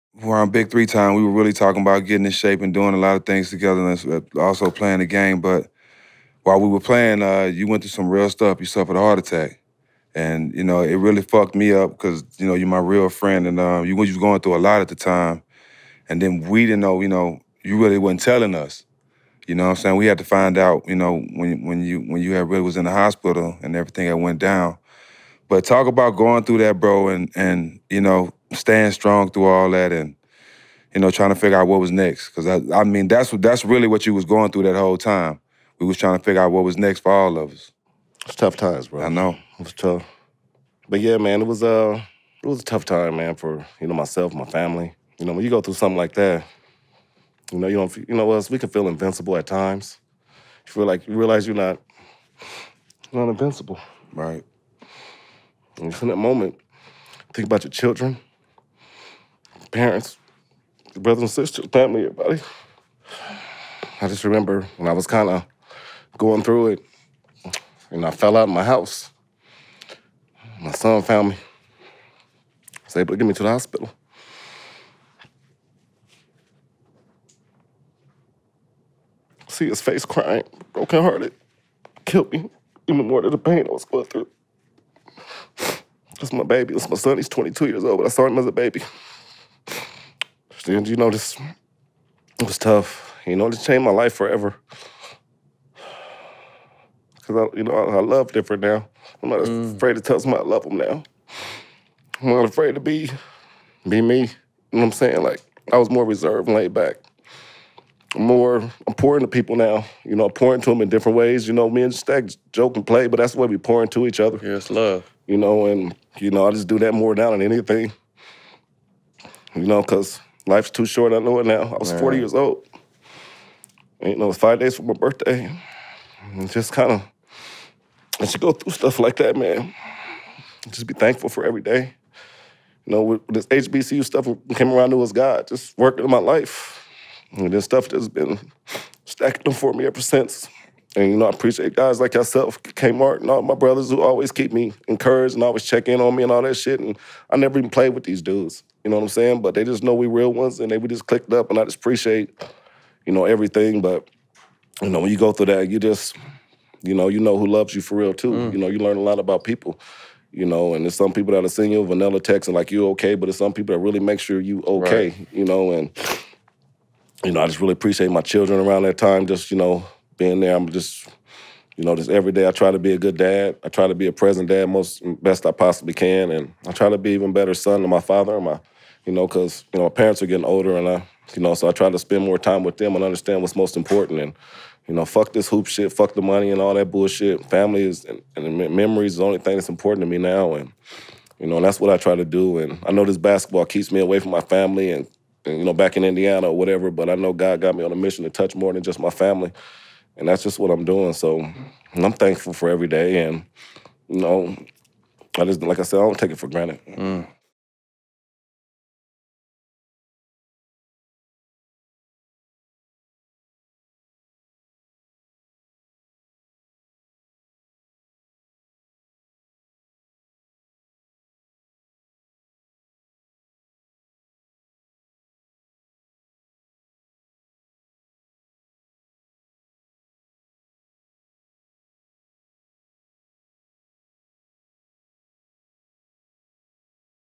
“I’m Not Afraid To Tell People I Love Them Now." | Bonzi Wells Opens Up On 2017 Heart Attack
12-year NBA vet Bonzi Wells emotionally opens up about his 2017 heart attack that changed his life.